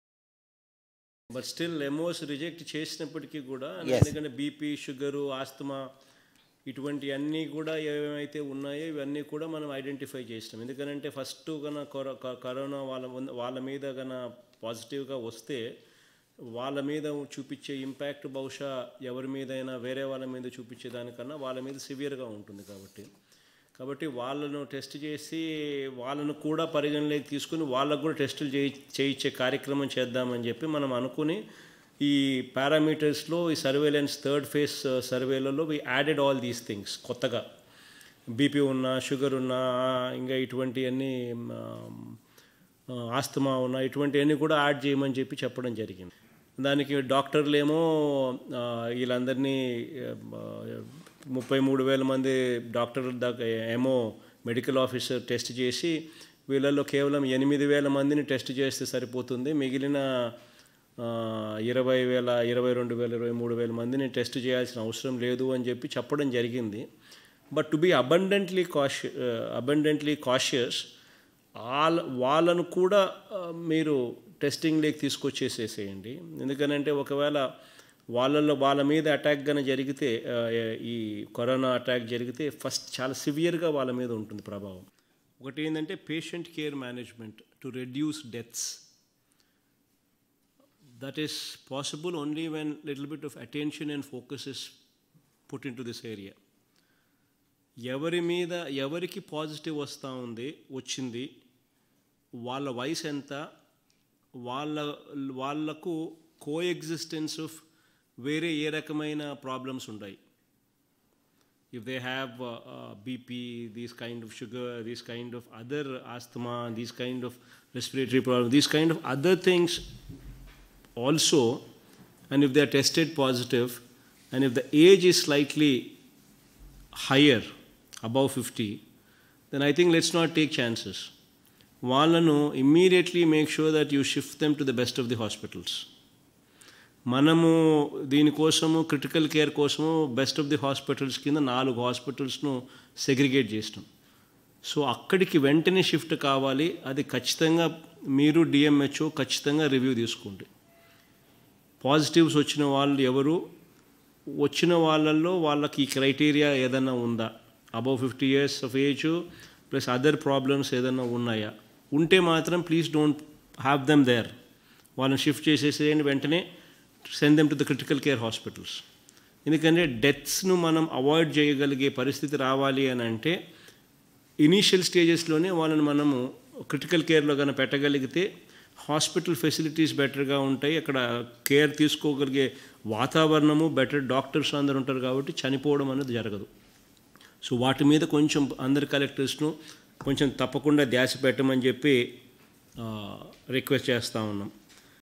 CM Jagan on Covid-19 patient care mangaement.
CM Jagan on Covid-19 patient care mangaement review meet with AP Officials at CMO in Amaravati.